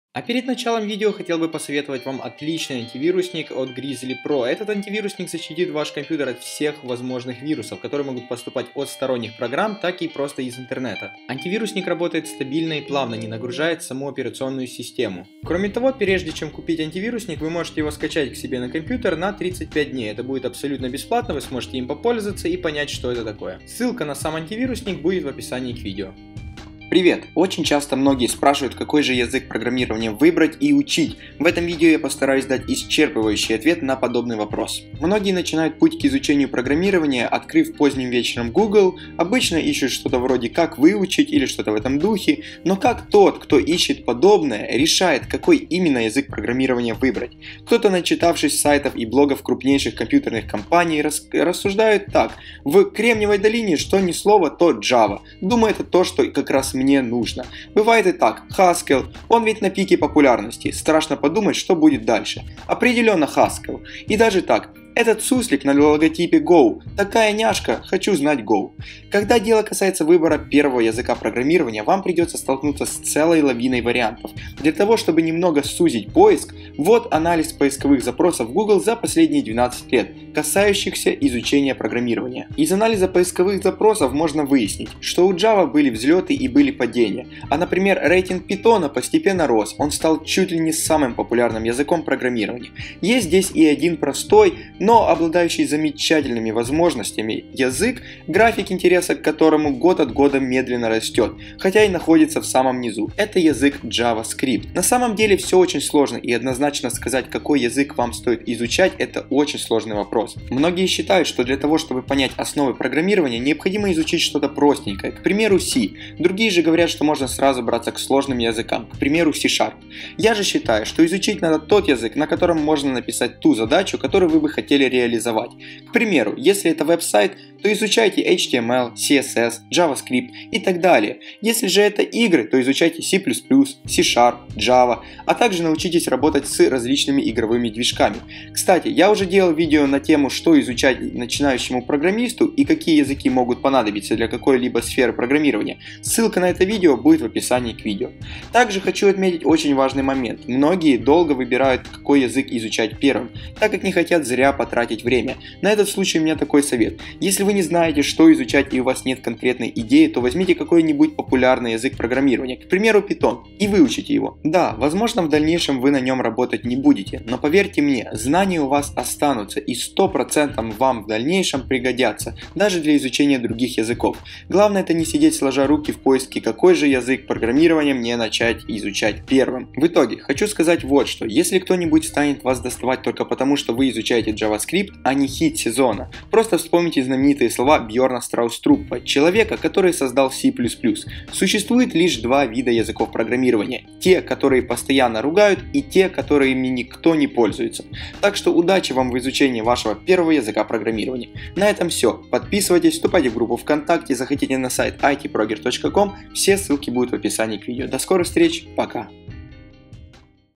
Какой язык программирования учить?
Какой язык стоит первым изучать? С чего начать начинающему программисту, какие языки стоит изучать. В этом видео я расскажу вам какие языки стоит изучать начинающему программисту.

* Кошельки WebMoney:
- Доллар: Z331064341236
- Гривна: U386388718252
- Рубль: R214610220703